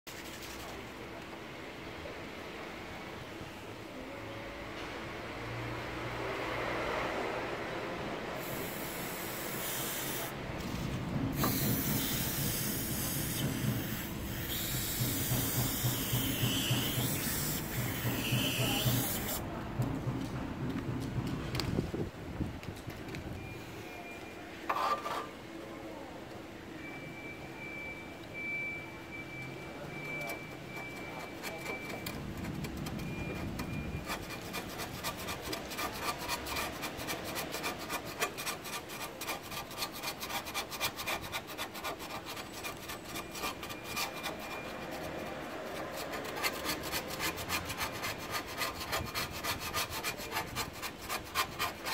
Decapant (585) industrial strength stripper, removal of paints and varnishes from all surfaces.
Free of CMR compounds, can be used to remove plastic residue, silicone gaskets, or pigment that accumulates in plastic injection moulds. Removes graffiti. Use on tile, concrete, wood, metal, glass, stone, brick, marble. Free of CMR compounds, acid-free. May be used cold on all materials: wood, metal, cement and on most types of paint. A professional product made available for general use.
Does not harm metals or wood. Easy-to-use.